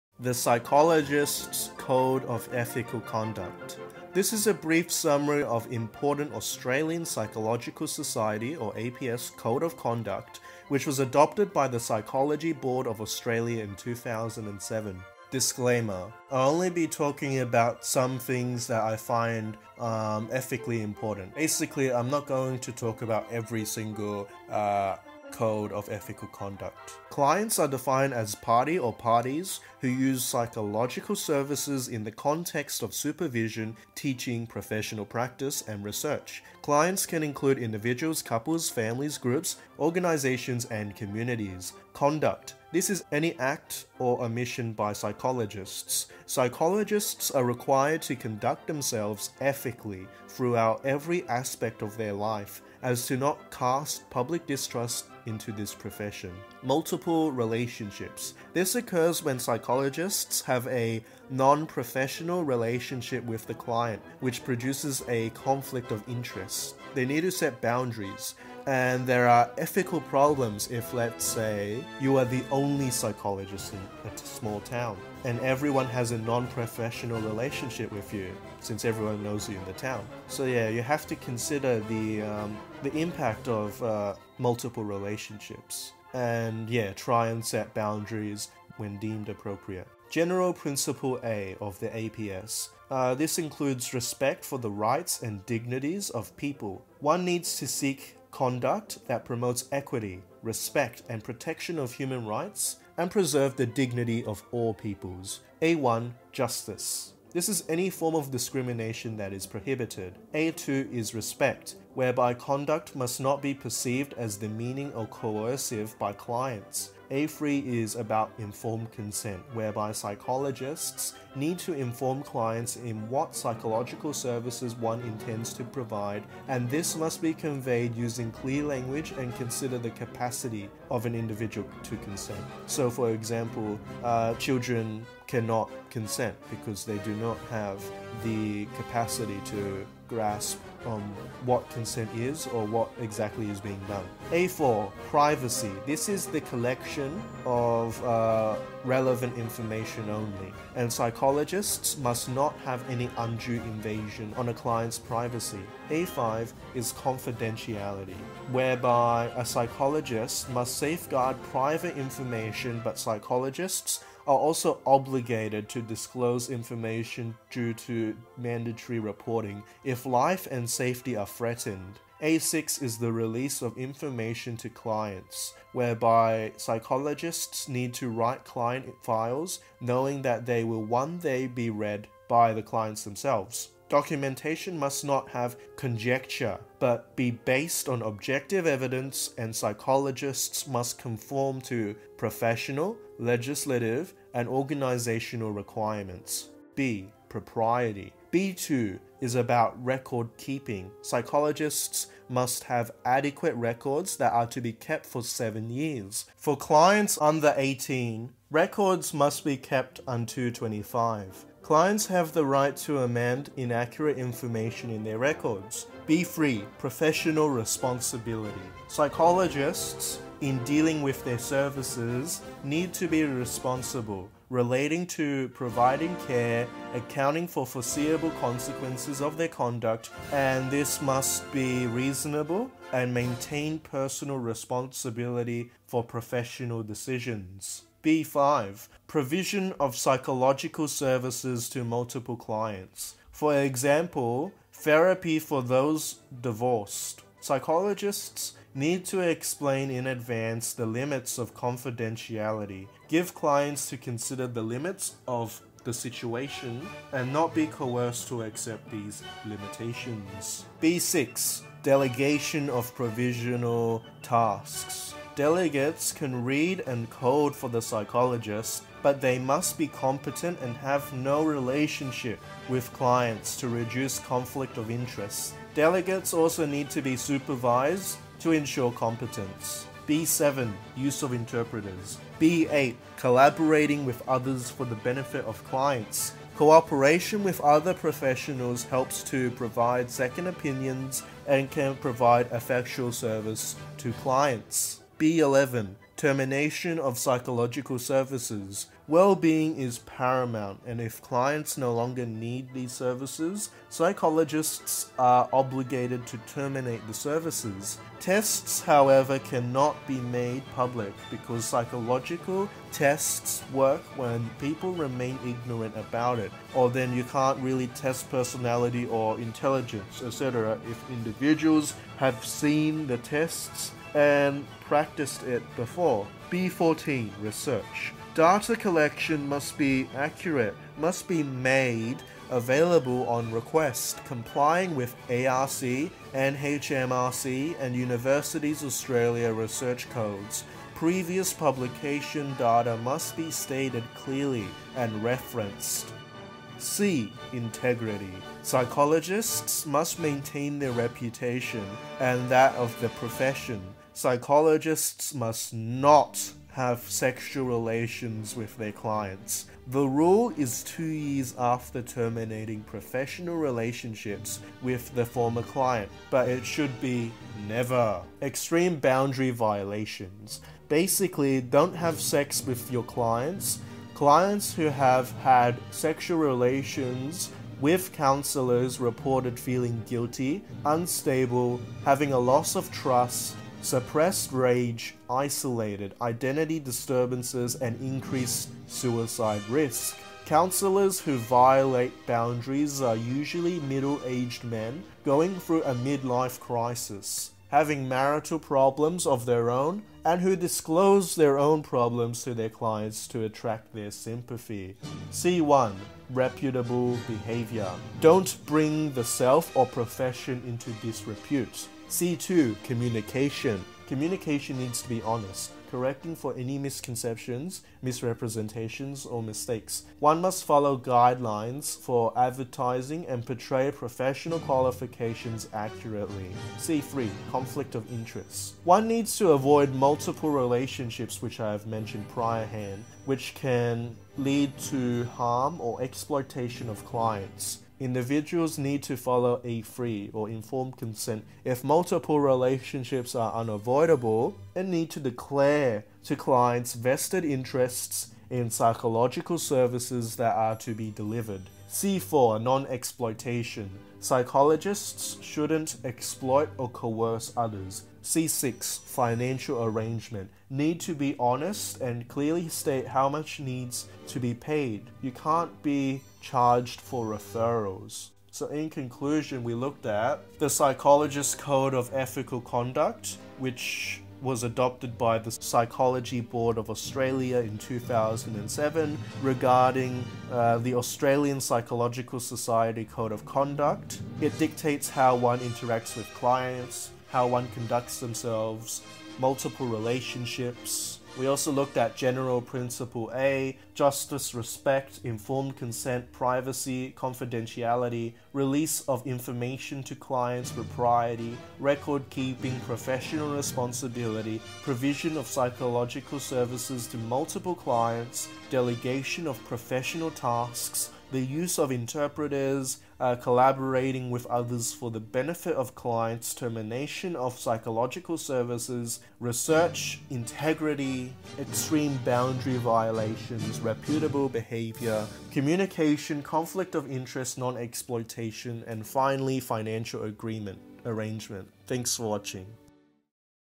Psychologist's Codes of Ethical Conduct (APS)
This is a brief summary of important Australian Psychological Society (APS) Code of conduct which was adopted by the Psychology Board of Australia in 2007.